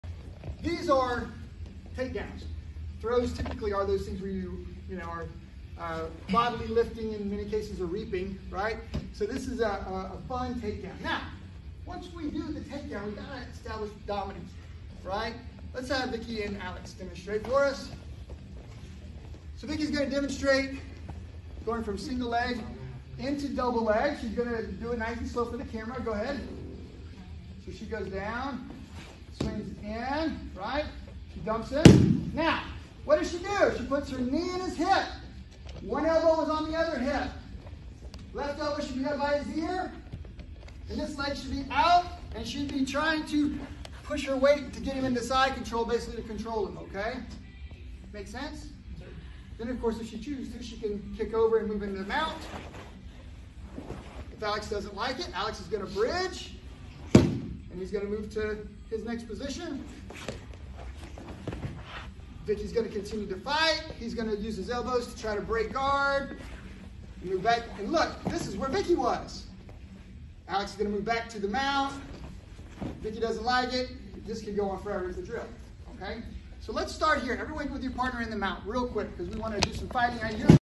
Why do we IMMOBILIZE after the TAKEDOWN? (Position/Transition Drills)
In this insightful discussion, Renshi explains the critical importance of immobilizing your opponent after securing a takedown. This step is essential for maintaining control and preventing counterattacks.

By understanding and applying these principles, you'll enhance your ability to control and dominate your opponent after securing a takedown. We always love hearing from you!